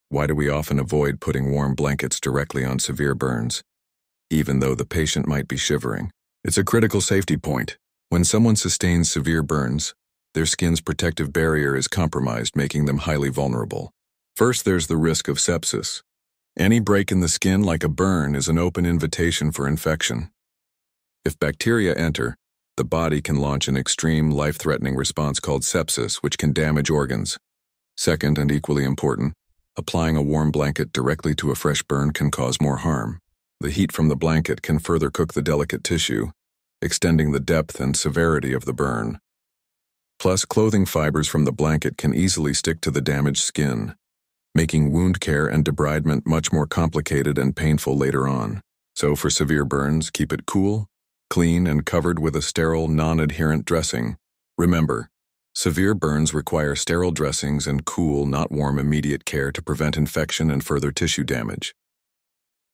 Why You Should NEVER Put Warm Blankets on Severe Burns!
Did you know that putting a warm blanket on a burn patient can actually worsen their injury?

Here’s why it’s a critical mistake:

⚠️ Severe burns destroy the skin's barrier, making infection (like sepsis) a serious risk.
🔥 Warm blankets can cook the tissue further and cause more damage.
🧵 Blanket fibers may stick to the burn, making wound care extremely painful and complicated.

Instead, cover burns with sterile, cool, non-stick dressings — not heat!

Perfect quick learning for med/nursing students, EMTs, and first aid responders.